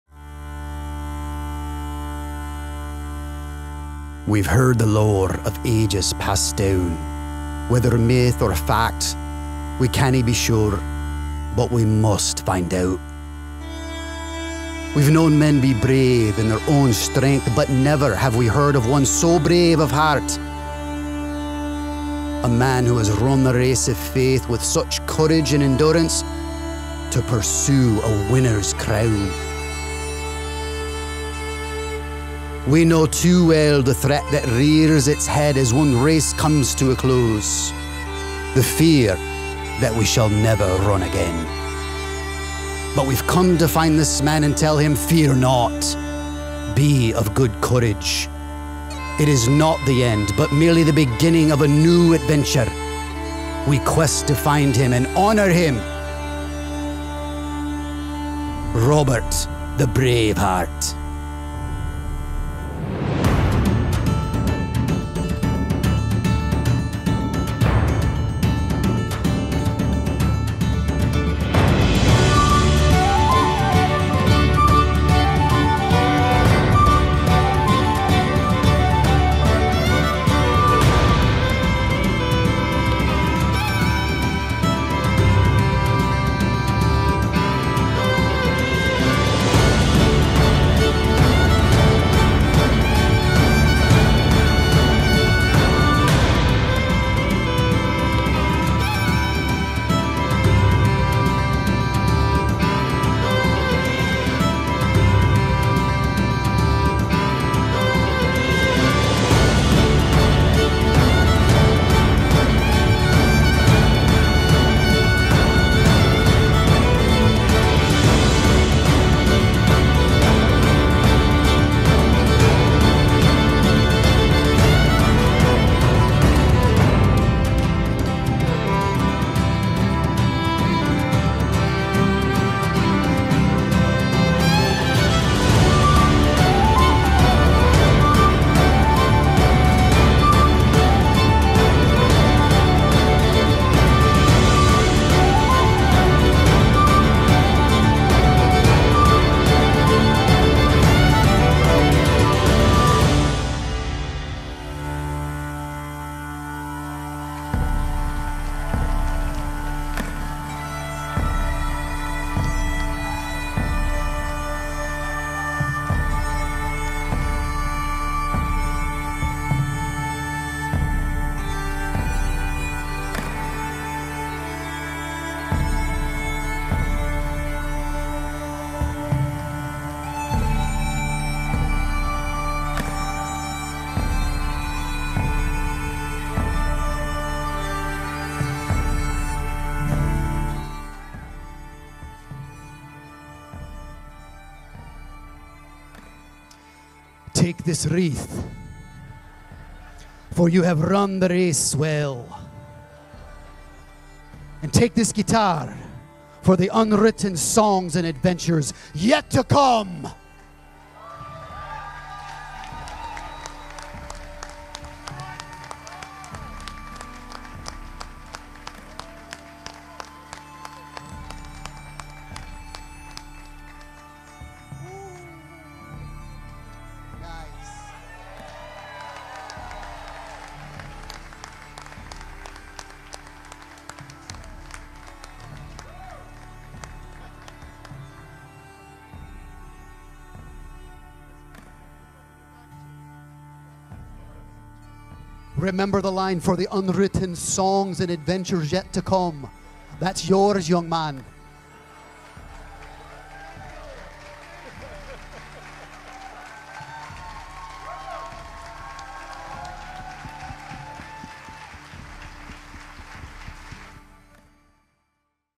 Robert the Brave Heart
They’ve heard the Lore of ages passed down. Whether myth or fact, they cannot be sure, but they must find out. They’ve known men to be brave in their own strength, but never have they heard of one so brave of heart. A man who has run the race of faith with such courage and endurance to pursue a winner's crown. They know too well the threat that rears its head as one race comes to a close—the fear that they shall never run again. But they’ve come to find this man and tell him, “Fear not! Be of good courage! It is not the end but merely the beginning of a new adventure!” They quest to find him and honor him—Robert the Brave Heart.